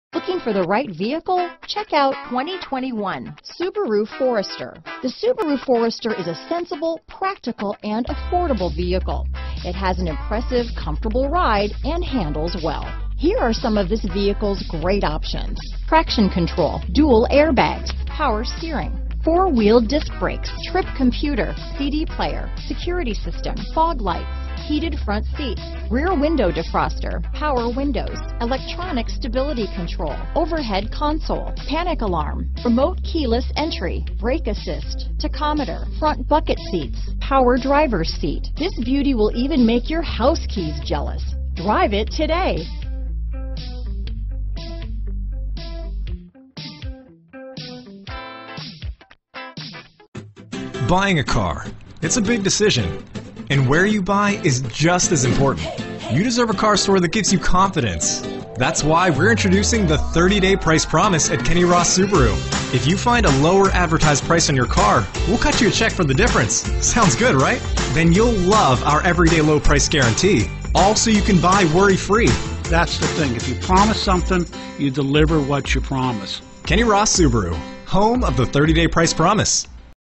2021 Subaru Forester Pittsburgh, Greensburg, Monroeville, Irwin, North Huntingdon PA 21U3415
Crystal White Pearl New 2021 Subaru Forester available in North Huntingdon, Pennsylvania at Kenny Ross Subaru. Servicing the Pittsburgh, Greensburg, Monroeville, Irwin, PA area.